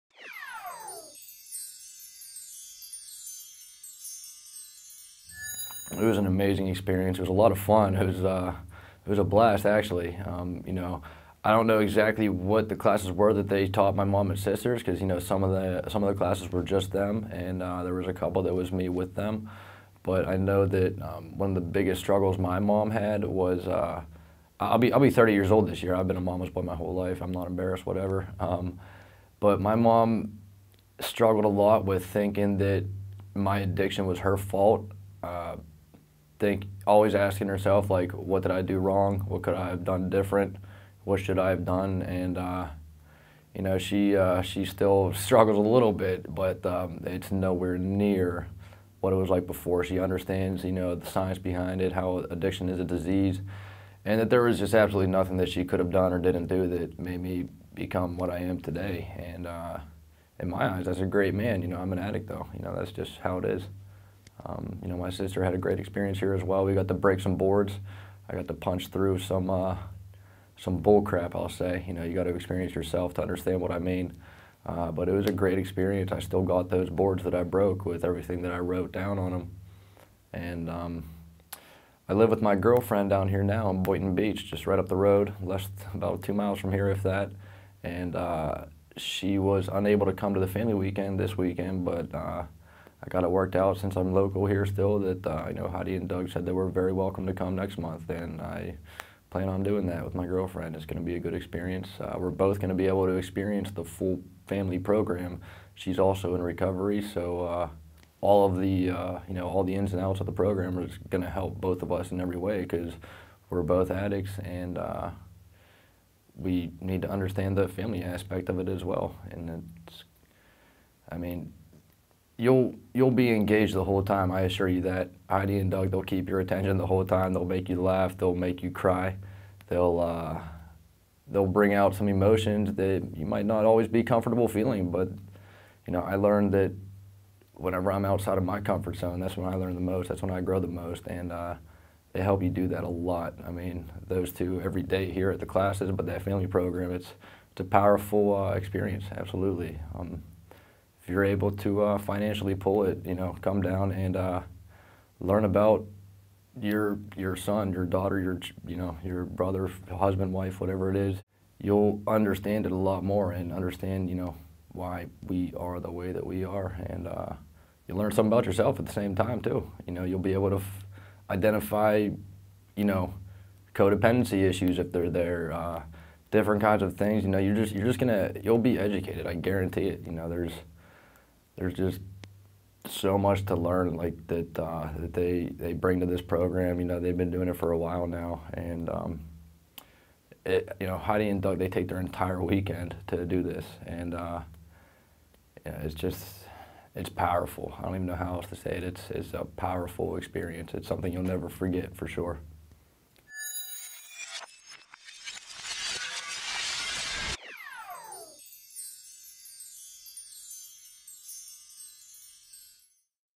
RJ’s Experience with The Family Program Testimonial - Palm Healthcare Review
RJ from Florida took part in The Family Program weekend and here is his testimonial on the journey they took with this program that teaches family and friends how to deal with loved ones who battle with an addiction.
Palm Healthcare Review.